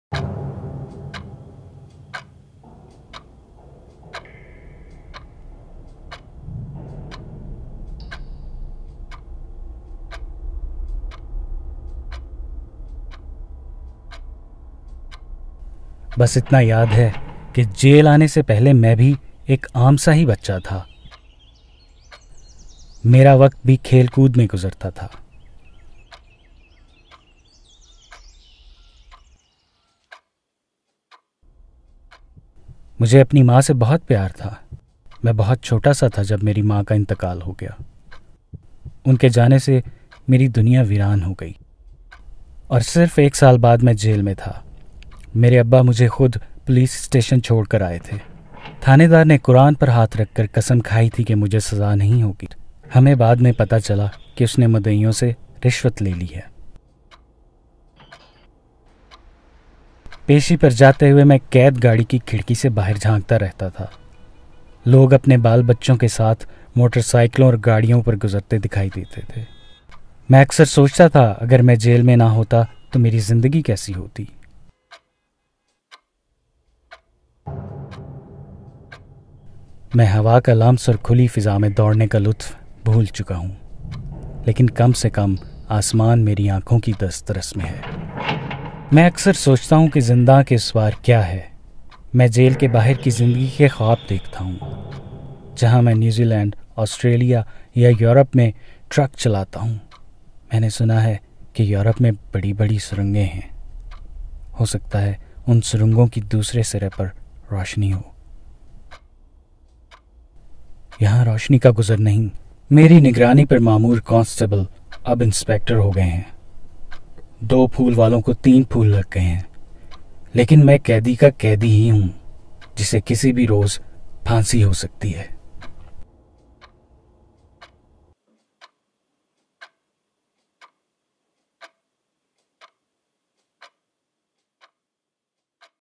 Letter from death row: ‘I often wonder what my life would have been like if I was not imprisoned’
Muhammad Iqbal was only 17 years old when he was sentenced to the harshest punishment of death. Today, he has spent over 20 years on death row, in contravention of Pakistan's domestic and international obligations.